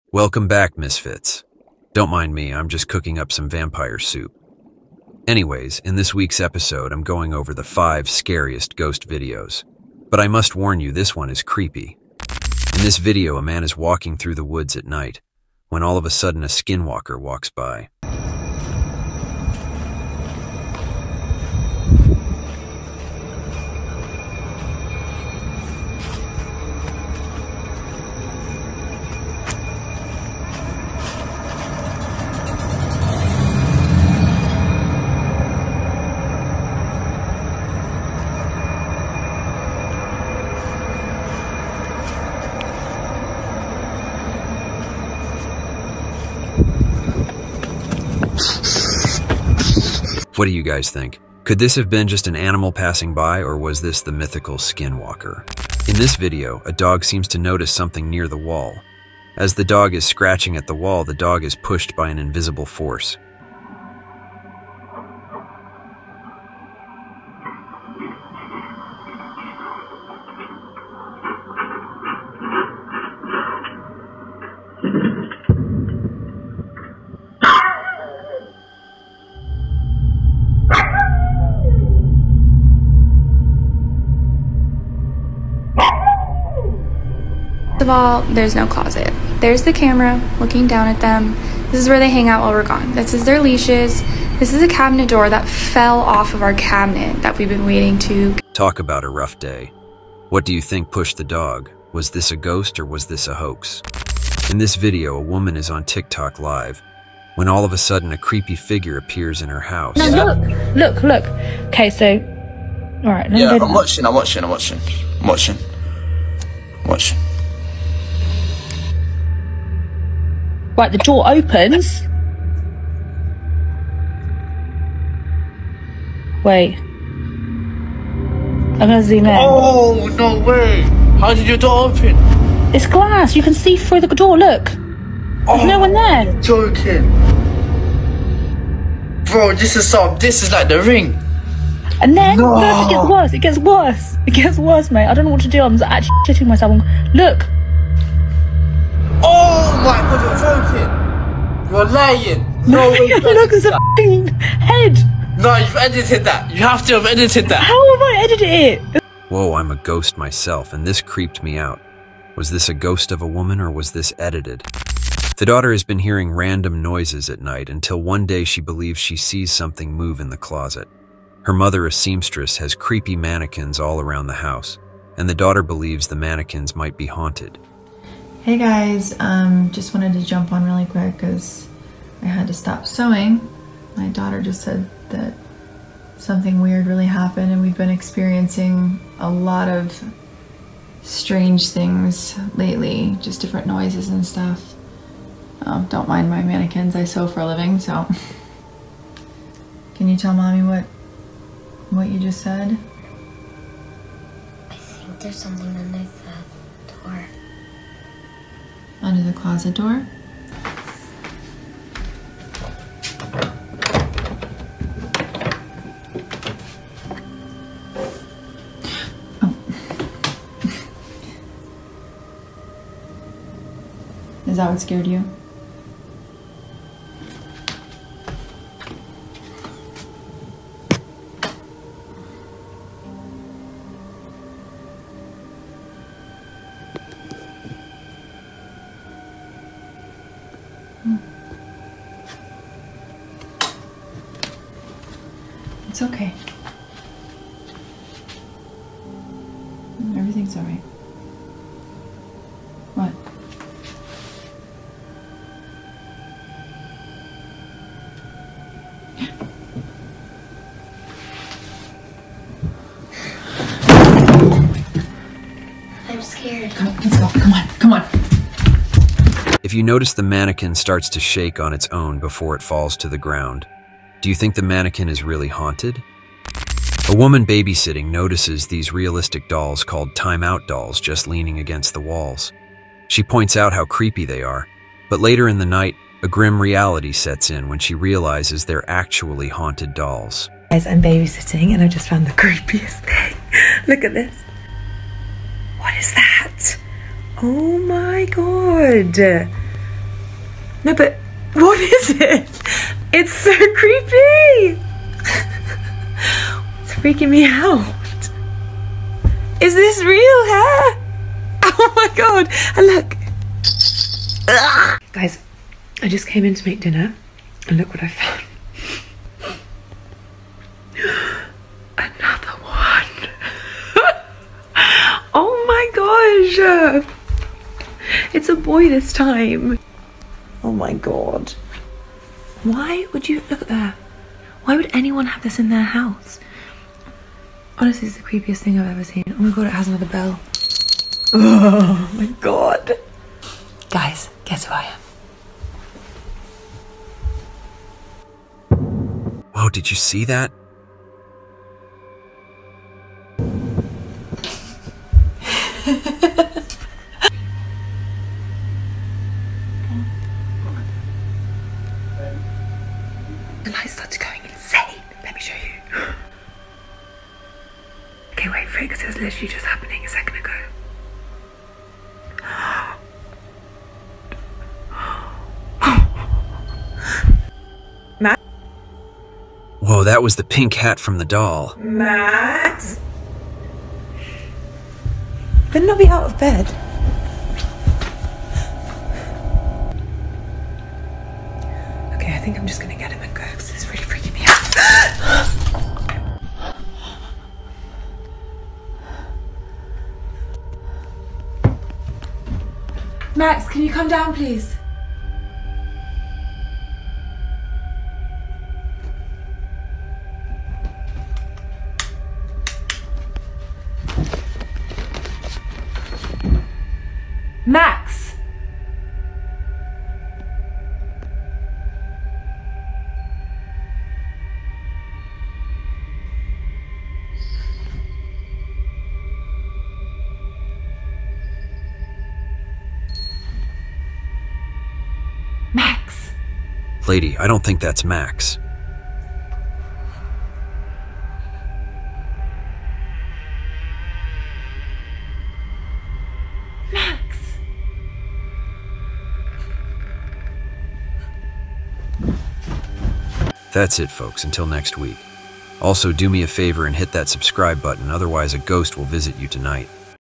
5 Scary GHOST Videos That Will Traumatize You! | Animated
In this video, we'll delve into a series of spine-chilling ghost encounters. Ranging from a man walking into a skin walker deep into the woods. Next a dog finds a ghost in his home and what happens next on camera is shocking.  To a ghost caught on Tiktok live as two tiktokers on live when all of a sudden the door opens up a ghost is caught on camera. Next is haunted mannequins that have been haunting the house, one is caught on camera moving. The final video is haunted doll or scary huanted dolls caught on camera as you can see the haunted dolls moving.